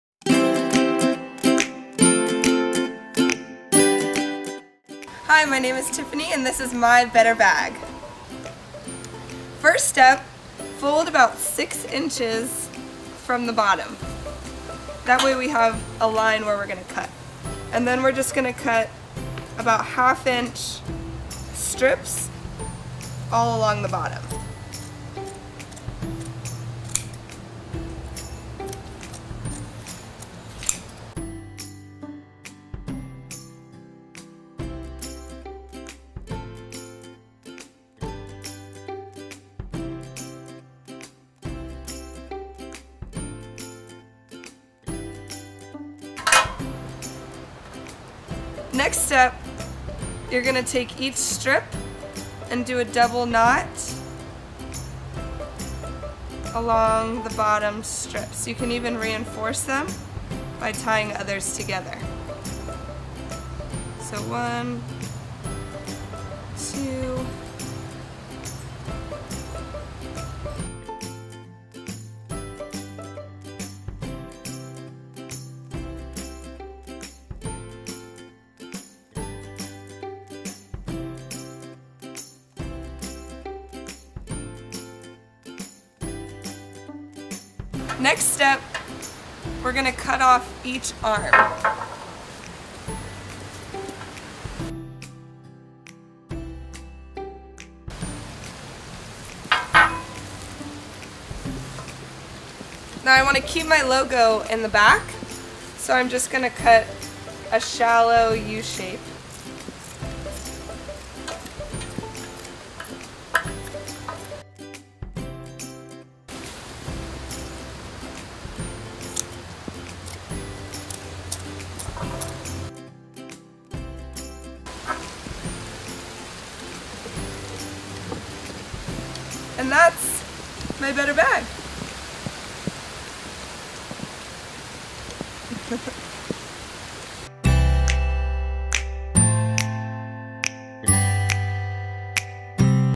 artinaction Better Bag Challenge - How to make a Better Bag
Art in Action is a global initiative that challenges persons from all around the world to GET UP, GET OUT and Take Action against plastic pollution using an artistic approach. Bahamas Plastic Movement teamed up with The Ocean Project to bring
you the ArtInAction World Oceans Day Better Bag Challenge!

For information on how to enter the Better Bag Challenge